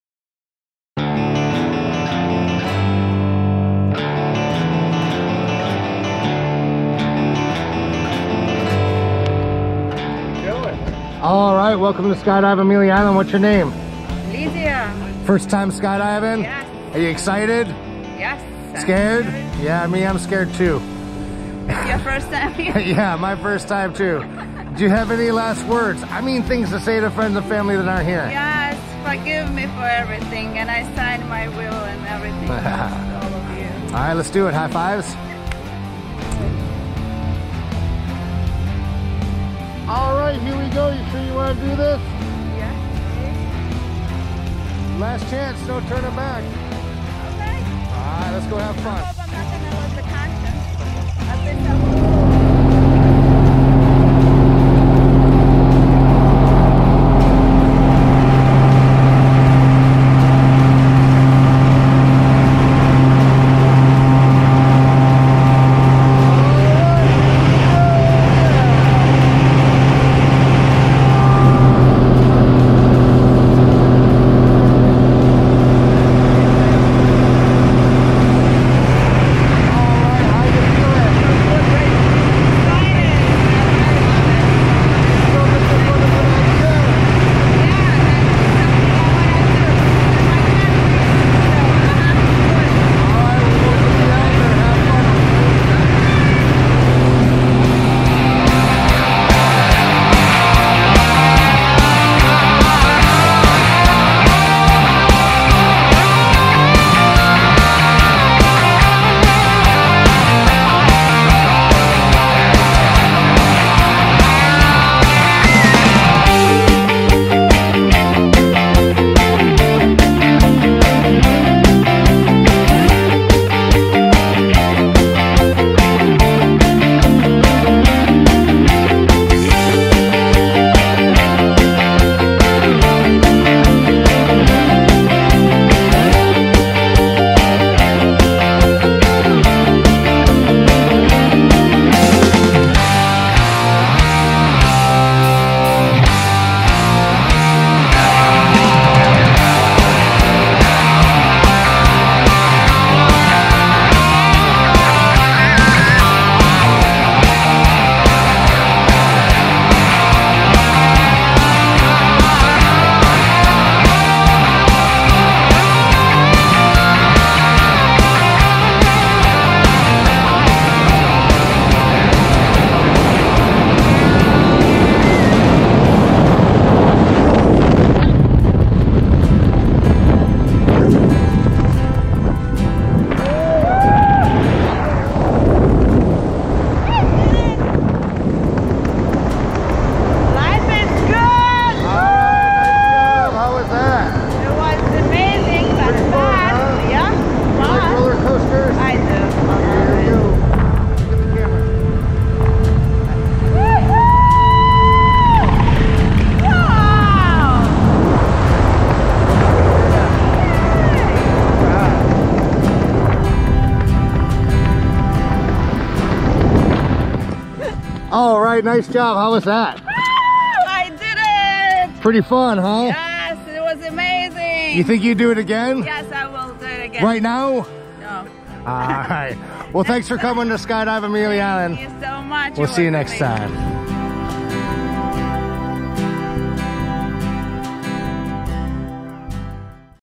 Lidiya - Skydive Amelia Island
Skydive over the beach at Amelia Island. Tandem Skydive near Jacksonville Florida - Lidiya